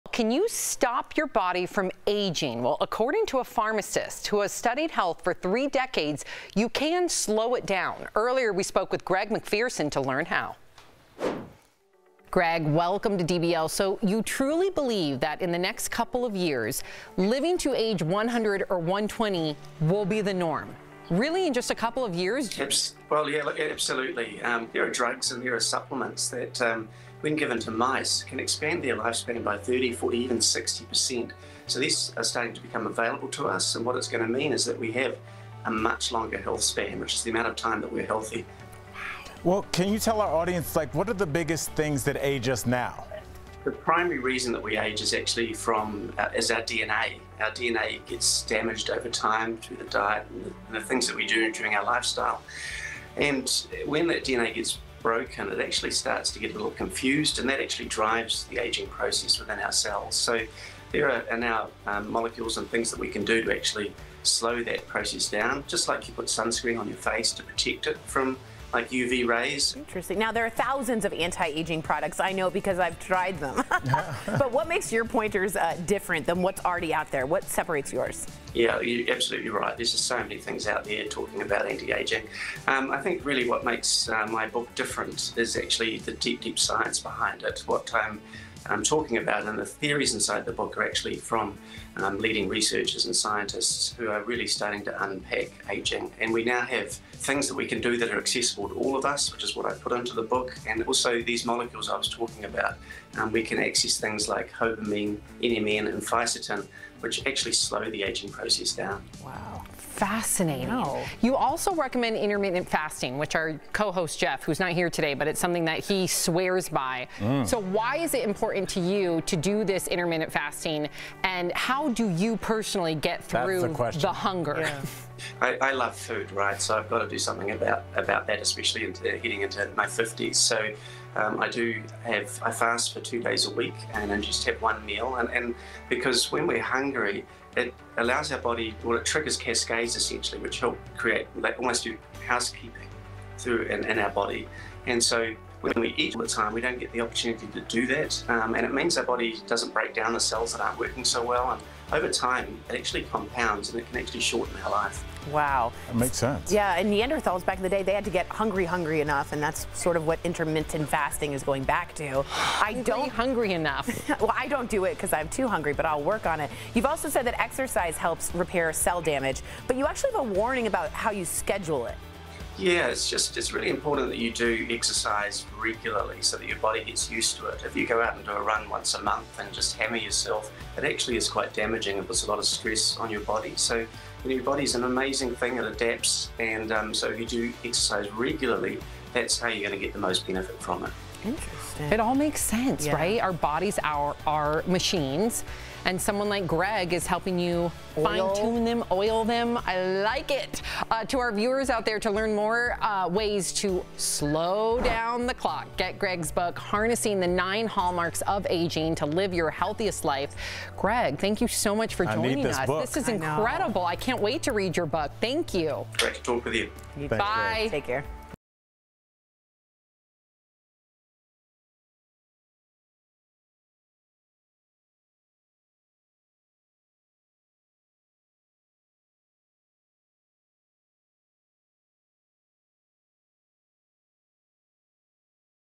Living to 120? Pharmacist & Author Greg Macpherson on What Ages Us, How to Slow the Process
Can you stop your body from aging?

According to pharmacist and author Greg Macpherson, who has studied health for three decades, it is possible to slow the process.

Monday - Friday
12:30  p.m. - 1 p.m. ET
1:30 p.m. - 5 p.m. ET
7:30 p.m. - 8 p.m. ET